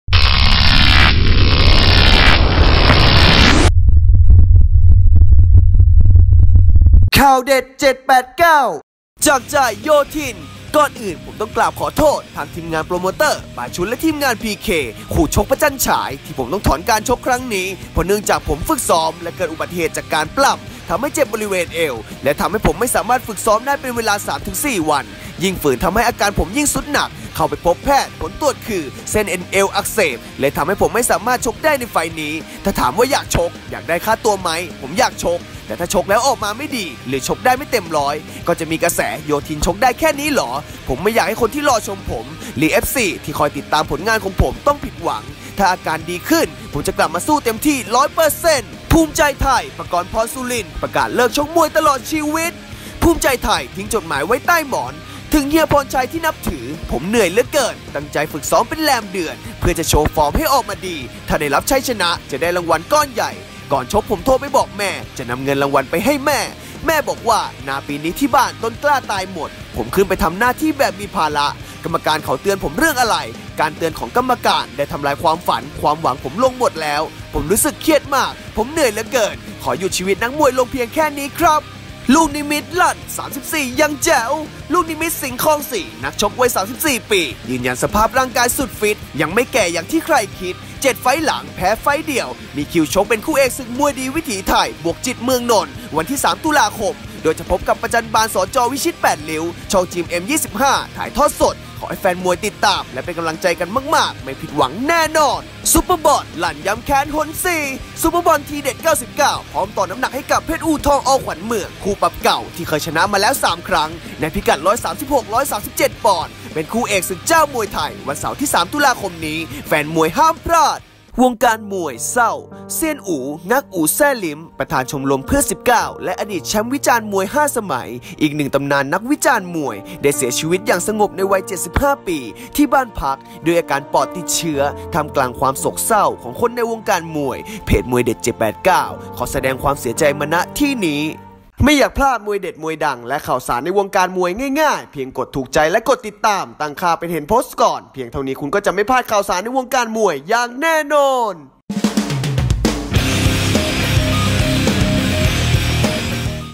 จากใจโยธิน ถอนชก พระจันทร์ฉาย !! | ข่าวเด็ด789 [30/09/63]
จากใจโยธิน ถอนชก พระจันทร์ฉาย !! , ภูมิใจไทย ปกรณ์พรสุรินทร์ ประกาศเลิกชกมวย , ลูกนิมิต ลั่น34ยังแจ๋ว, ซุปเปอร์บอล ลั่นย้ำแค้นหน4 ,วงการมวยเศร้า สิ้นแล้ว เซียนอู๋ | ข่าวเด็ด789 [30/09/63] สรุปข่าวเด็ด "ใช้แต่หูฟังไม่ต้องใช้ตาอ่าน"